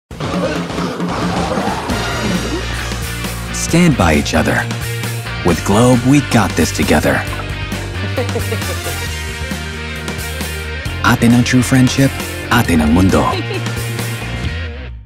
Show the love with SUR4ALL shareable data powered by Globe5G
Friends go better together! Show the love with SURF4ALL shareable data and GCash-exclusive deals powered by Globe’s 5G mobile connection.

💙 ASC Ref Code: G116P080321GS